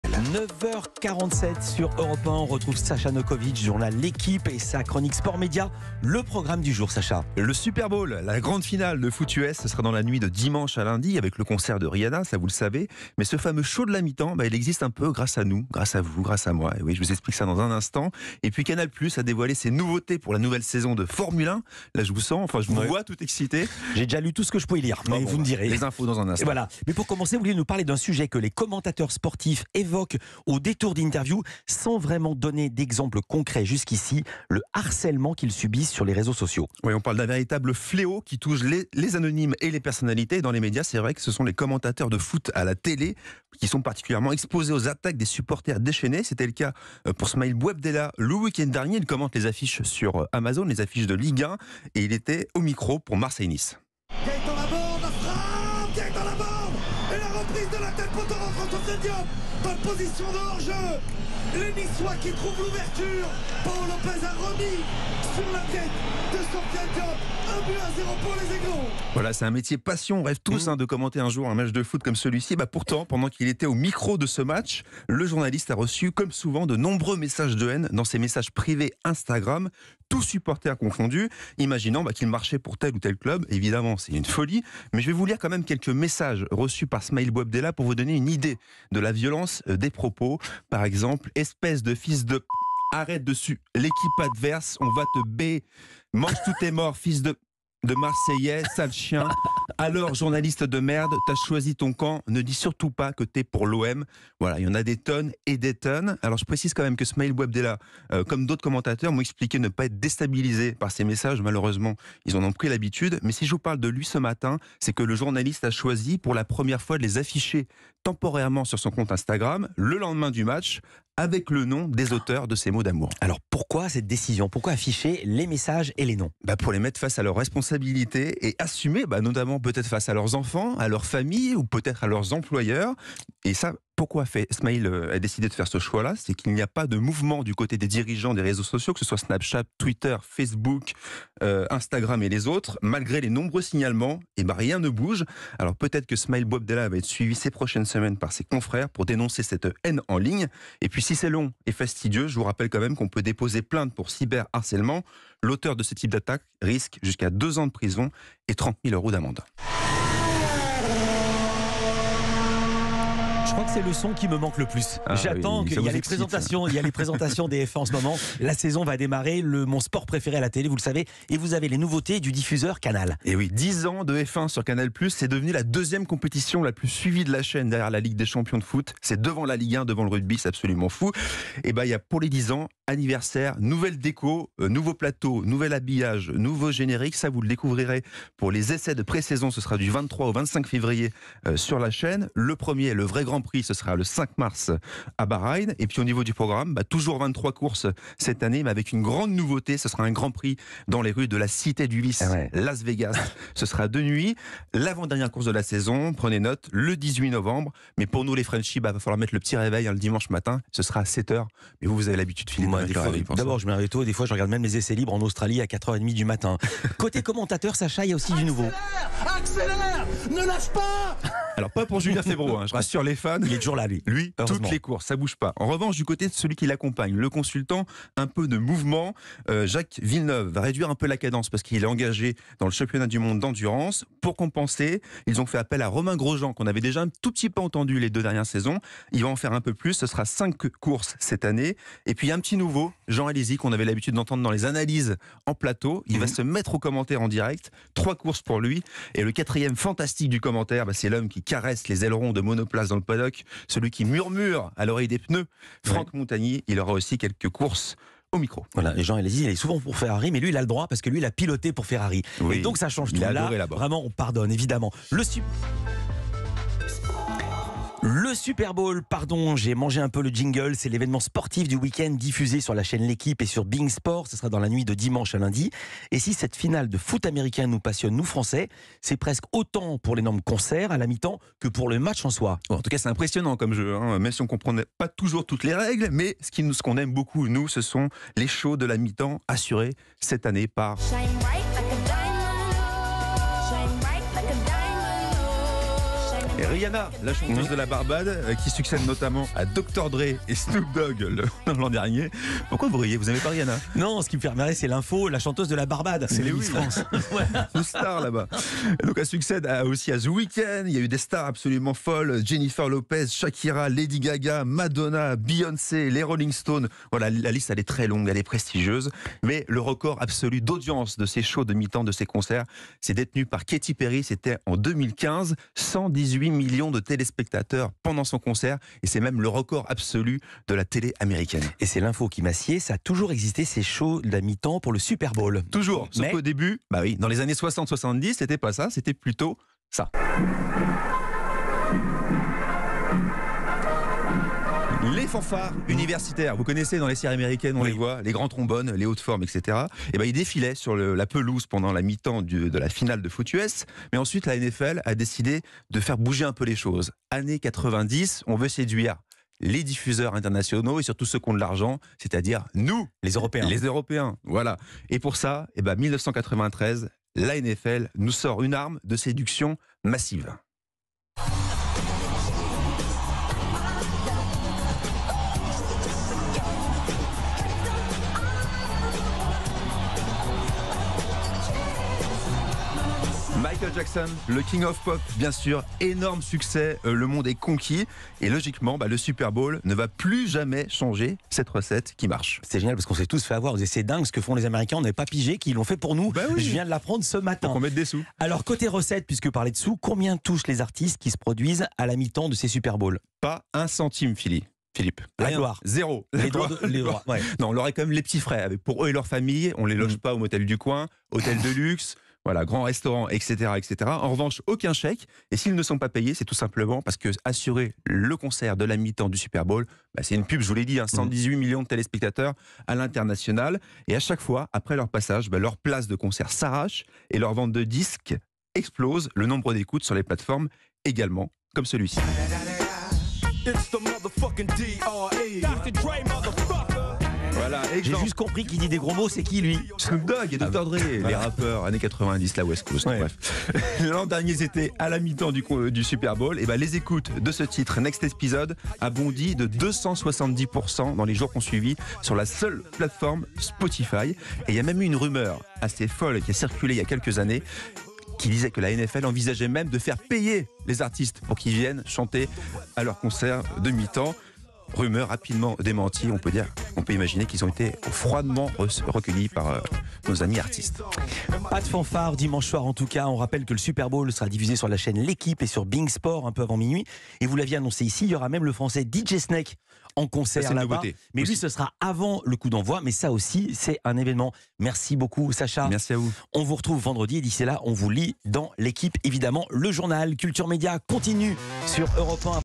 Retour sur le harcèlement des commentateurs sportifs
Chaque vendredi Sacha Nokovitch, journaliste média à "L'Equipe", décrypte l'actualité sportive par la lorgnette médiatique.